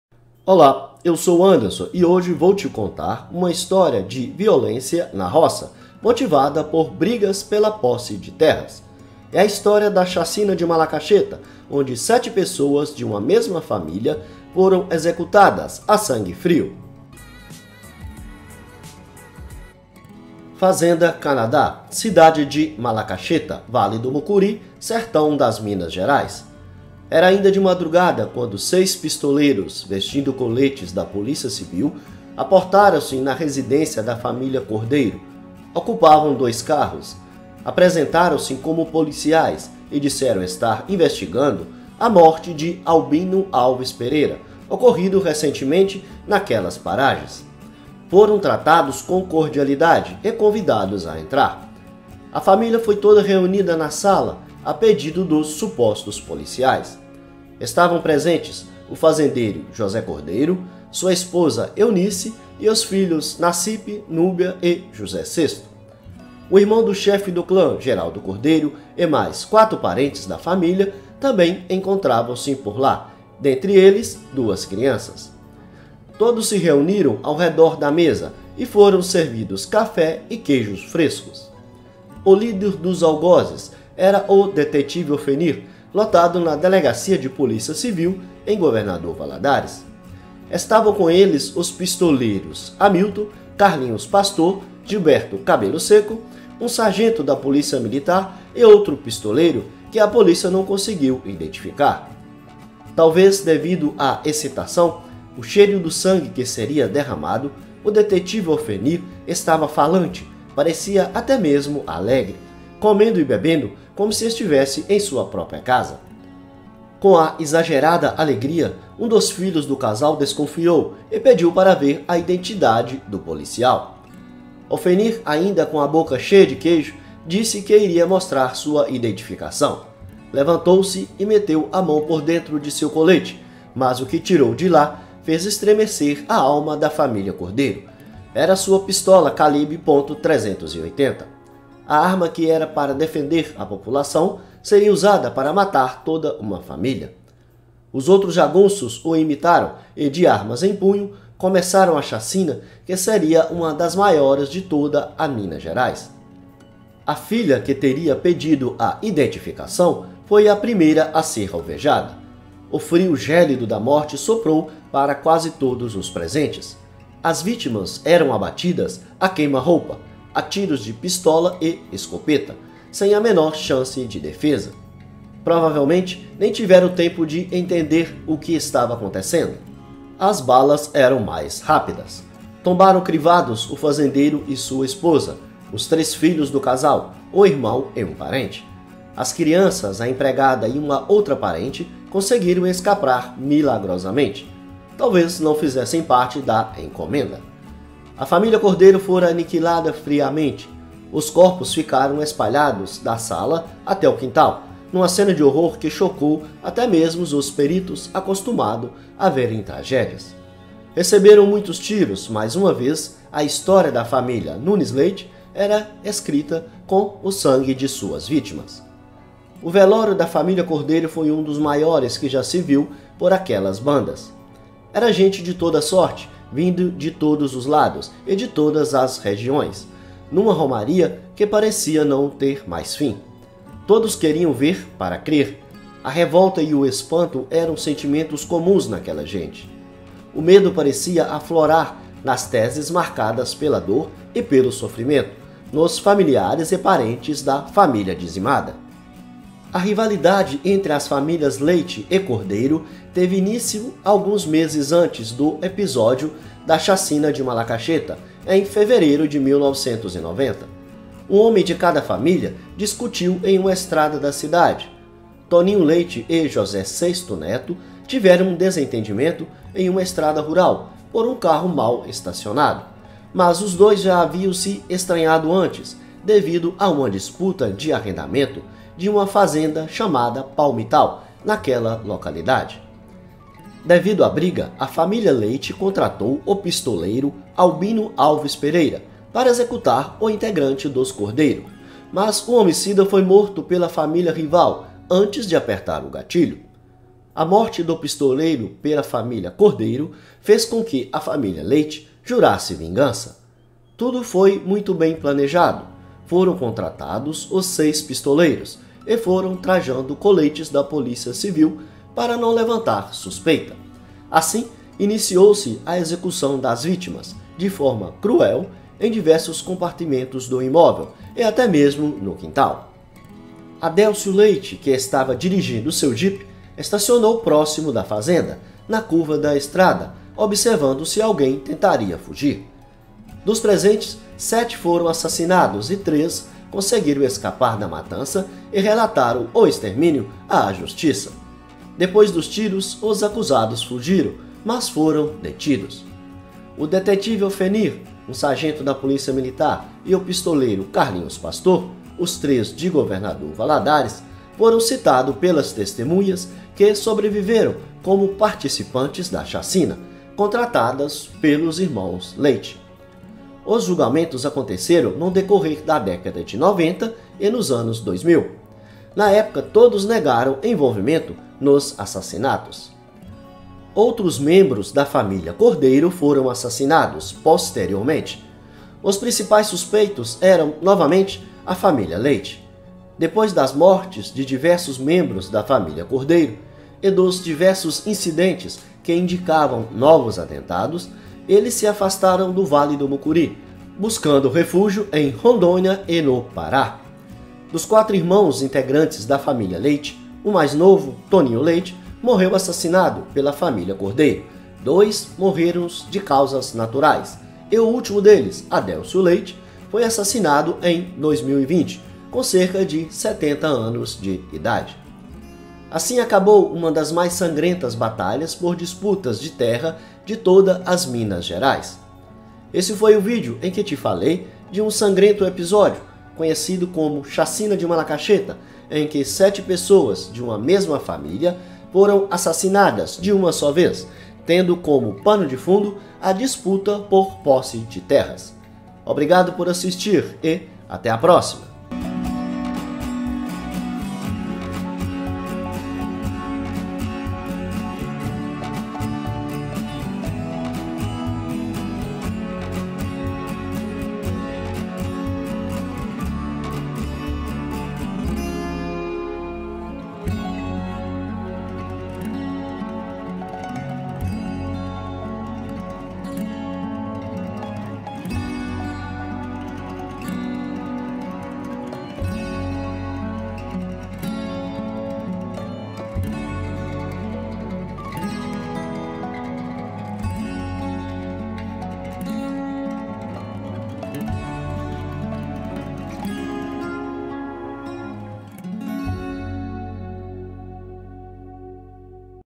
CHACINA na ROÇA: disputa por terra extermina 7 pessoas de uma mesma família!
Conta a história da Chacina de Malacacheta em que morreram 7 pessoas de uma mesma família, vitimadas pela disputa de posse de terra, na cidade do Vale do Mucuri chamada de Malacacheta, perdida nas montanhas de Minas Gerais. As vítimas eram da família Cordeiro que disputava uma fazenda com a família Leite. A família Cordeiro também abateu um membro da família Leite e que gerou uma guerra que culminou com a morte de Adelcio Leite no ano de 2020, aos 70 anos de idade. Assista esse vídeo e veja como a disputa por uma fazenda levou o caos e o medo a toda uma região do interior de Minas Gerais.